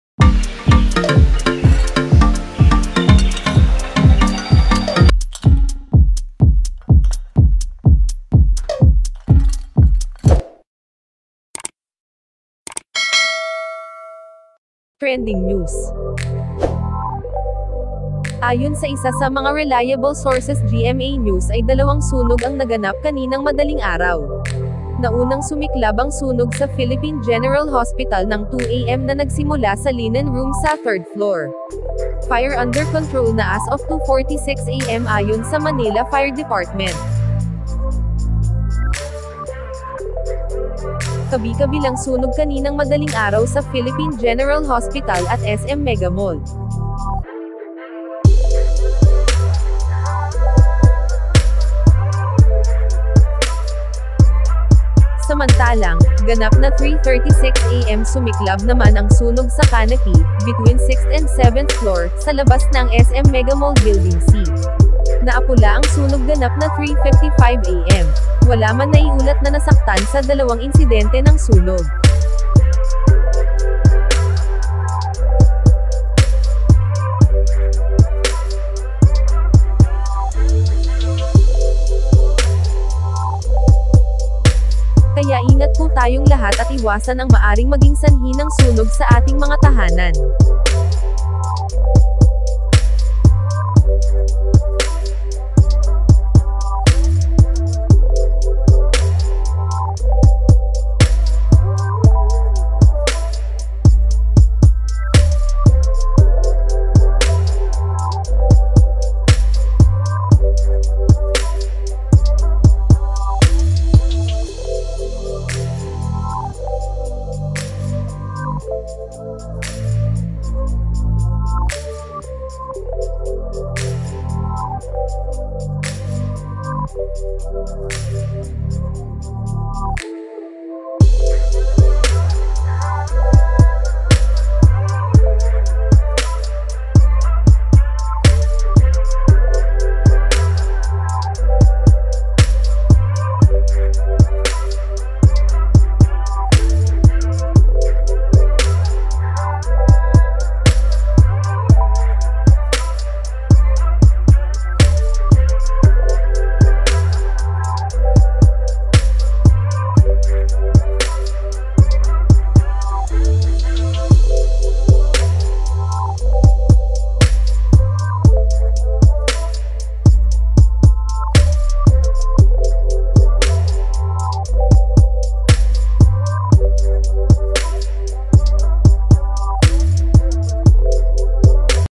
MGA KAGANAPAN SA SUNOG SA PHILIPPINE GENERAL HOSPITAL,MGA PASYENTE NAG-EVACUATE
@Trends and Vlogs